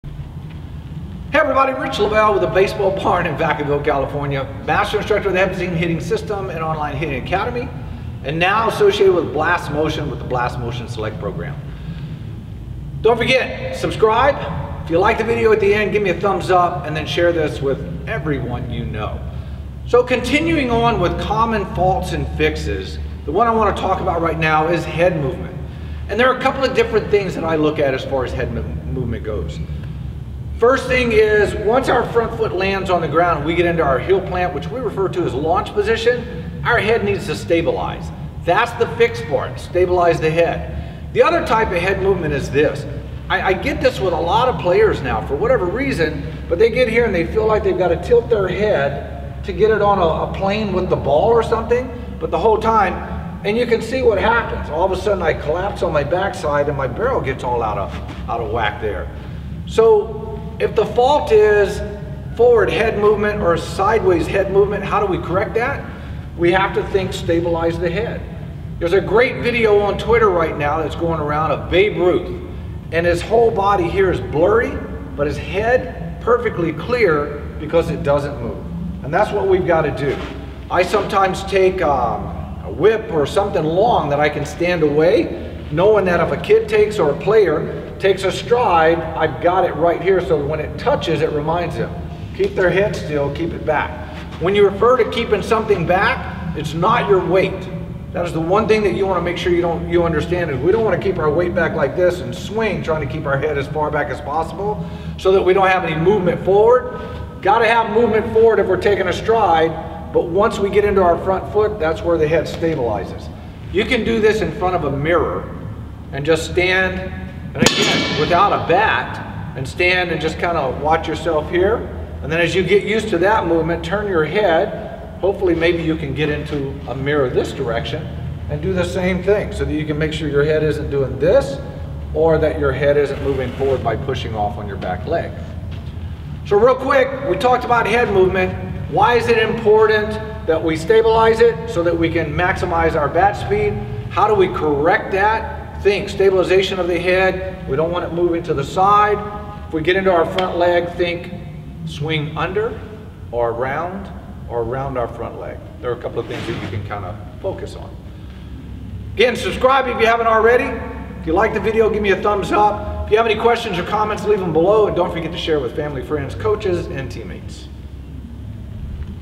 Common Faults & Fixes Series - Poor Head Movement - The Baseball Barn
Rich continues the common faults and fixes series with addressing head movement.

Click Here for Online Hitting Lessons, Video Analysis, Hitting Camps and lessons before 4p.m. through my Epstein Online Academy